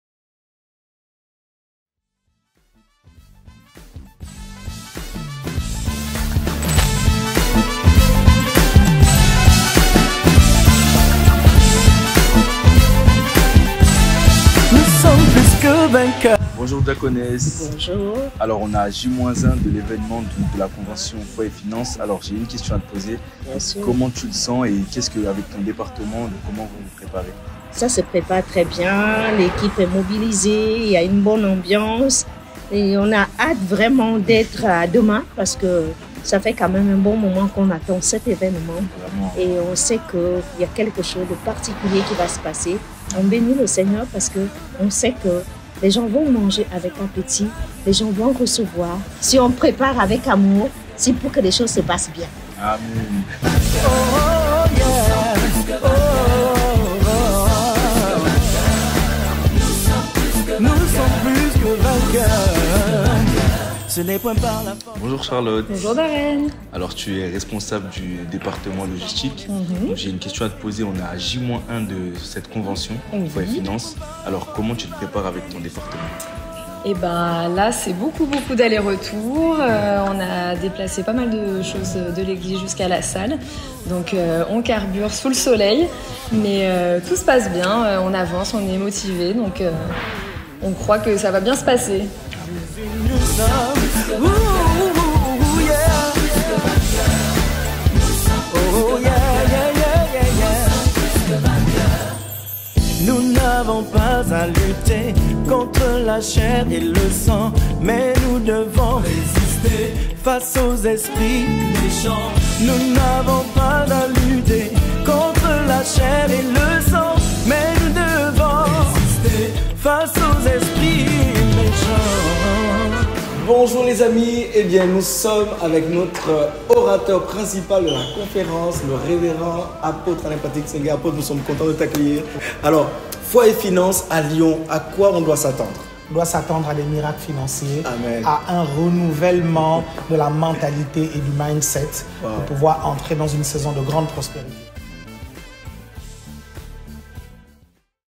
LE BEFORE
Nous vous emmenons en immersion dans les coulisses de notre évènement qui débute demain.  Les équipes travaillent dur pour la réussite de cette convention.
Si vous n'êtes pas encore inscrits nous vous invitons à le faire au lien suivant :